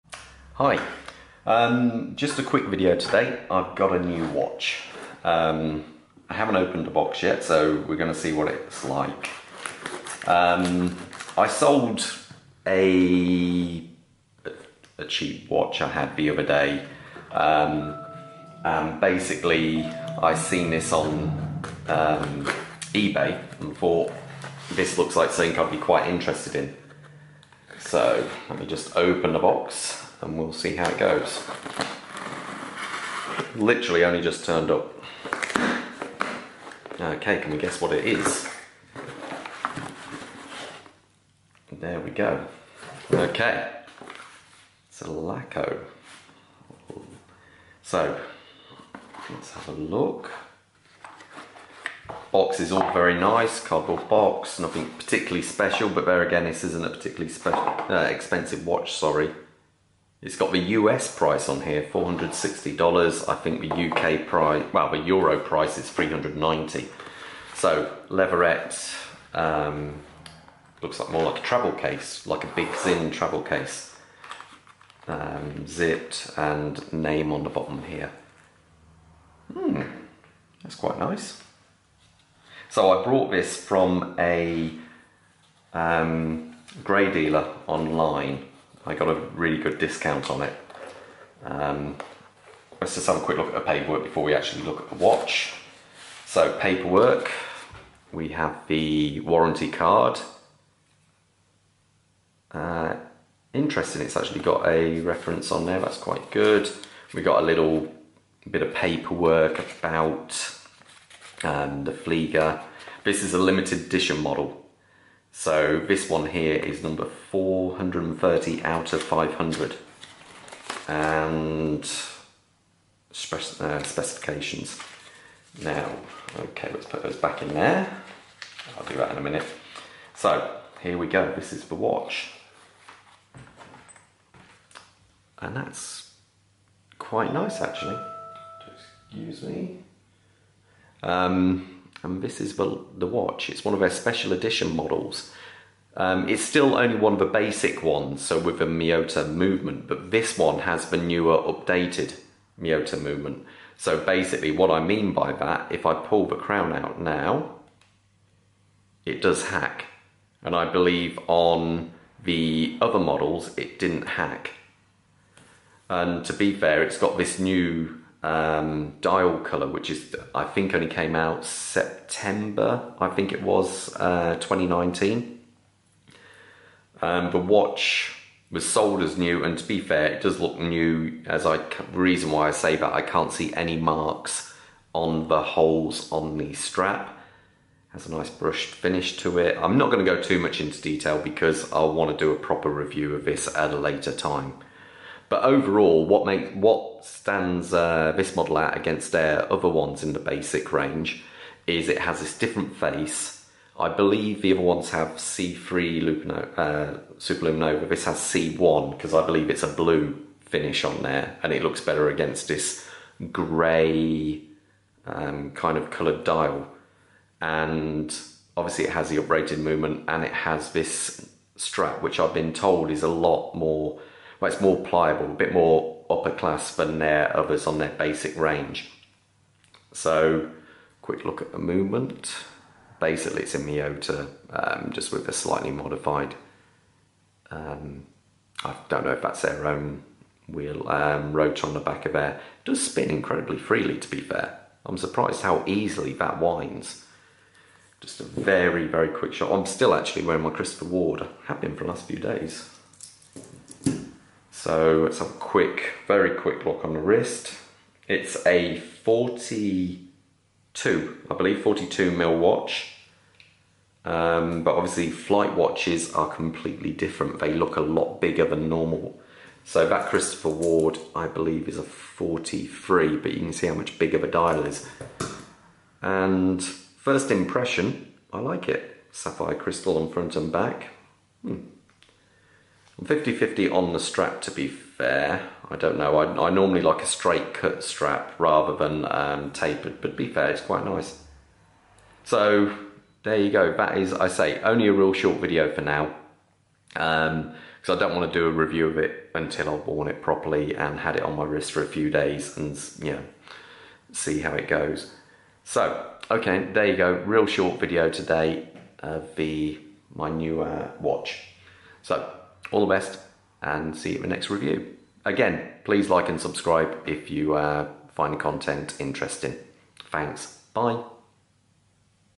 Laco Aachen Taupe Unboxing
Just a short video on the unboxing of my new Laco Aachen Taupe Le
Full review to come later